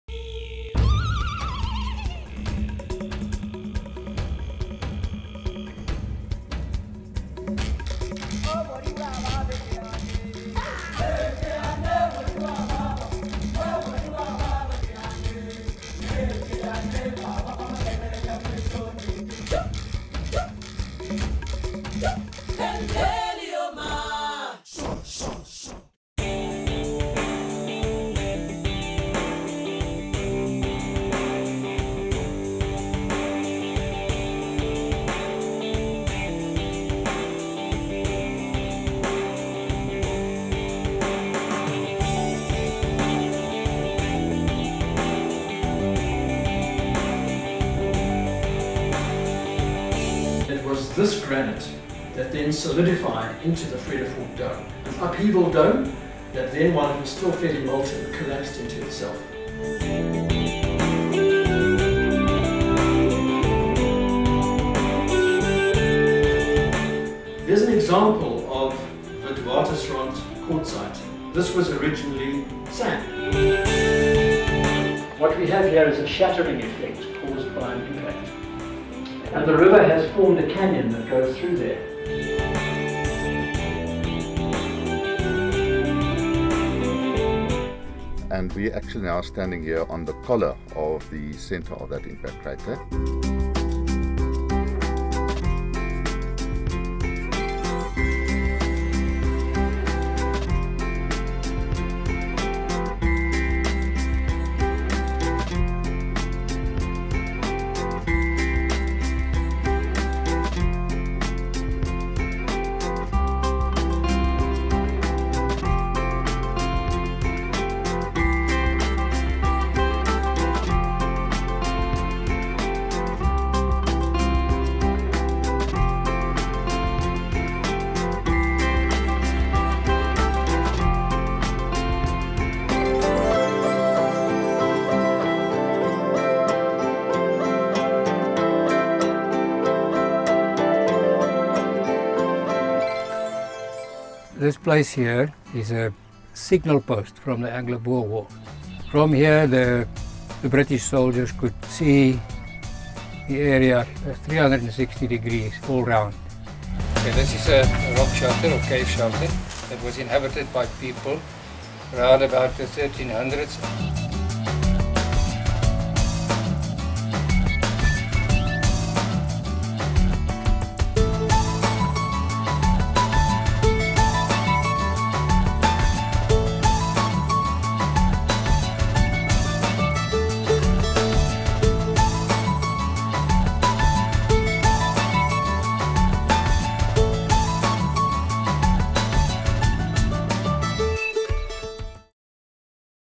Big Bang - Vredefort Dome
A brief look at the area where a meteorite hit the earth 2 billion years ago - the Vredefort Dome. Impressions of the African Heritage Epic team during the visit to the Dome Area.